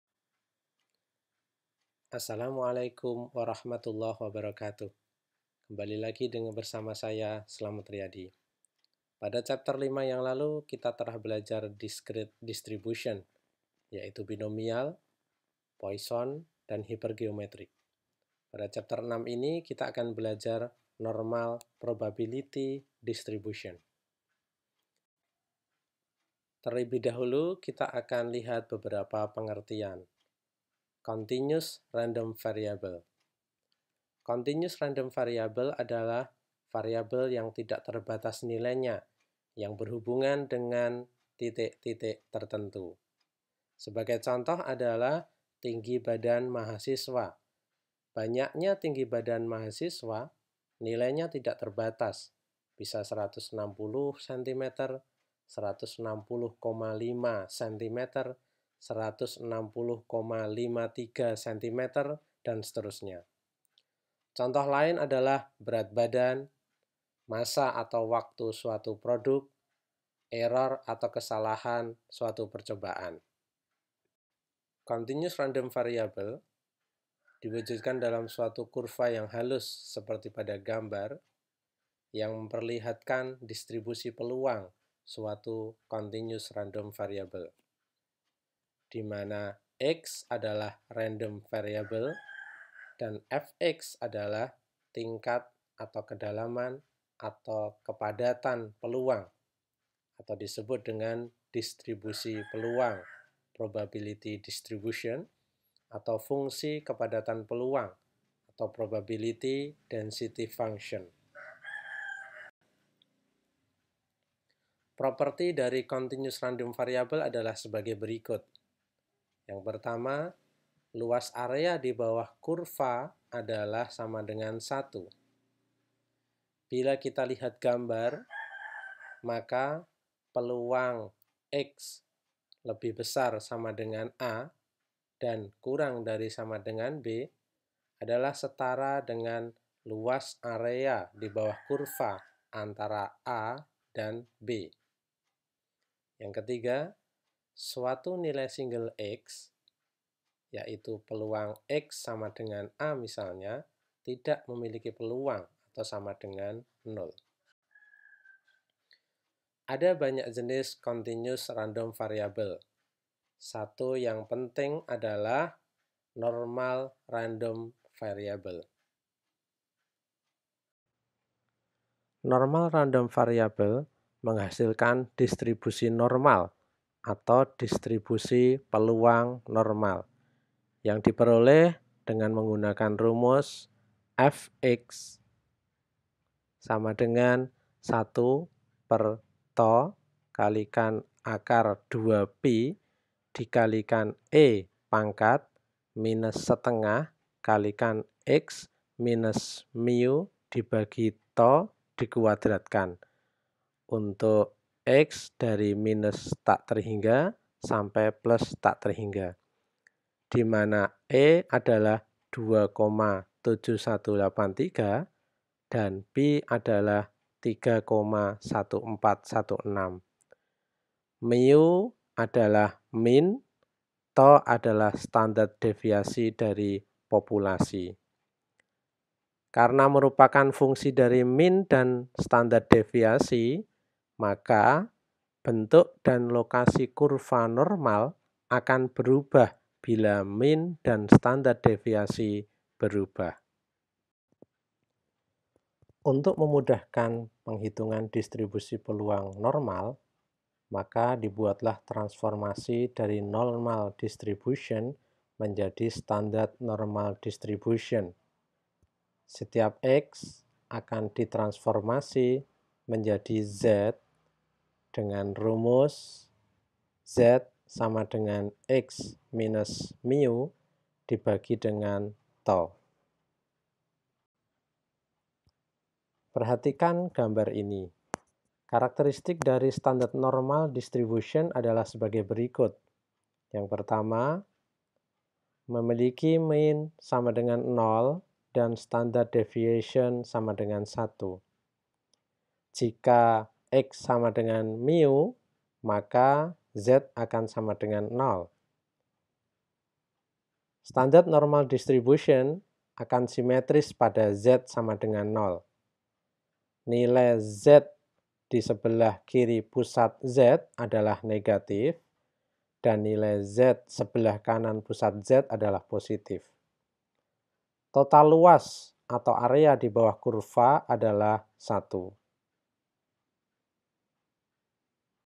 Ch-6 Normal Probability Distribution Part 1
Chapter 6 Normal Probability Distribution Part 1
Introduction to Probability and Statistic
Robert J. Beaver • Barbara M. Beaver • William Mendenhall
PPT dari Barbara M Beaver